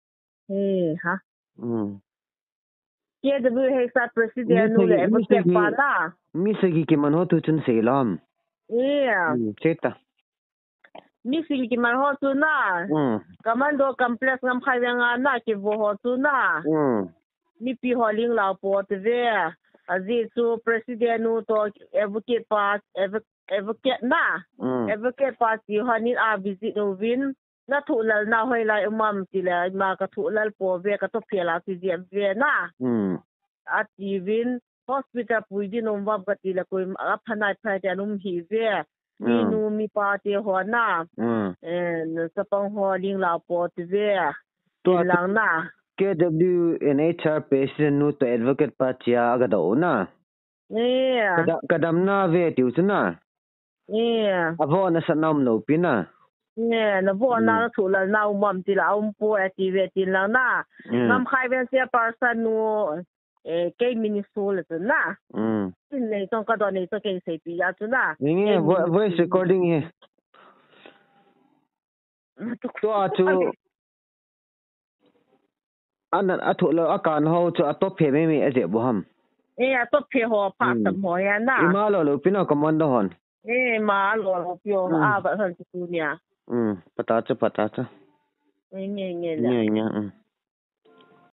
Eimi mi 7 kiman ho chung changthu
Mi 7 kiman ho chungchang a KWU&HR  hon USB jinu Tiger through a Nam mite ahin hetsah u

Tunilhah 8pm